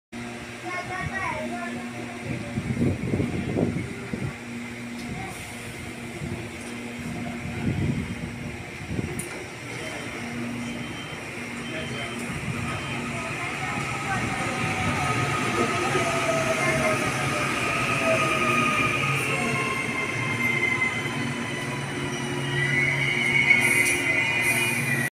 6 Carriage Electric Train Arriving At Puhinui
This Video was filmed on the 5th of March at Puhinui Train Station. This Train is operating a Southern Line Service from Papakura to Britomart Via Pamure and Glen Innes.